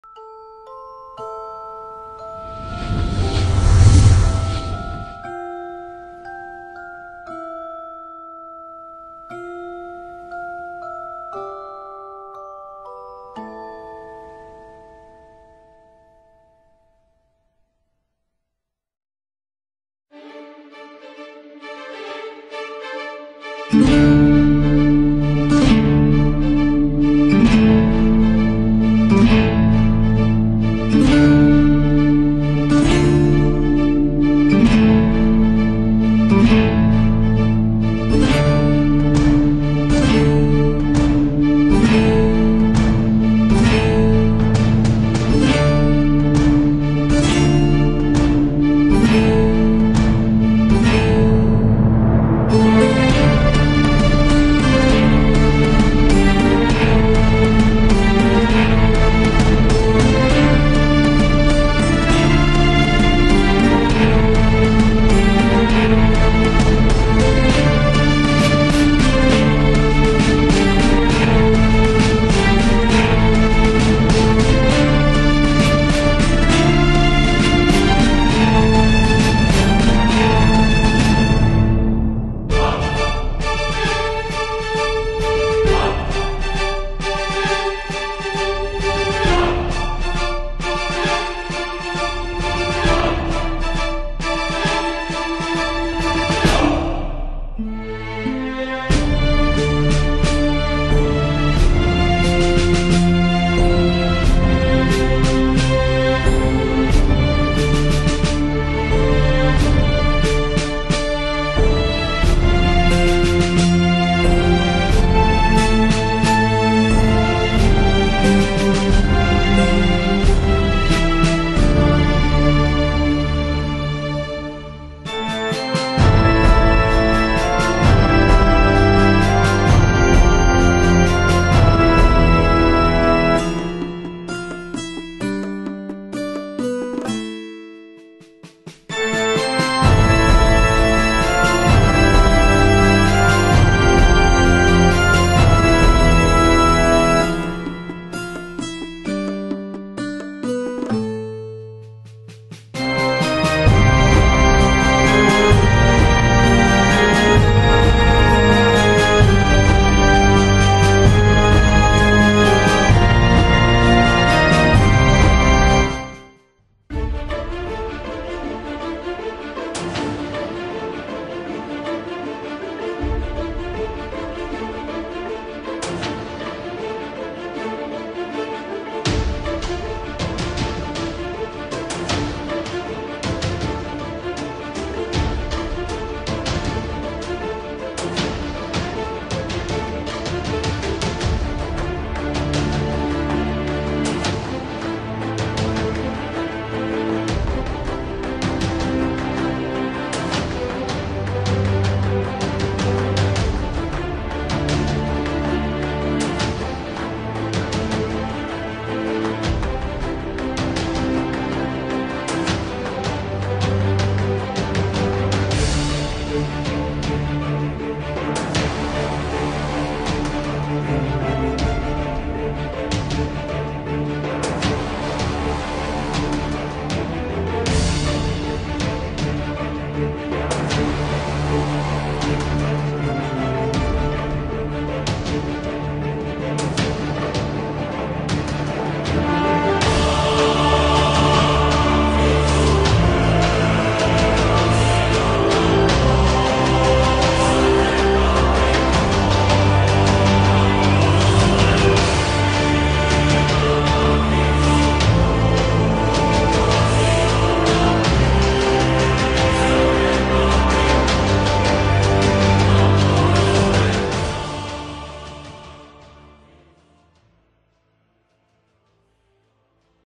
Airship Pirates World - Player built settlements on the Survival Server
This video shows all current major player built settlements on the Survival Server. Most are in some state of construction, some are barely started. But they are shown to give an idea of the scope of work carried out by the excellent builders of the AP Survival Server.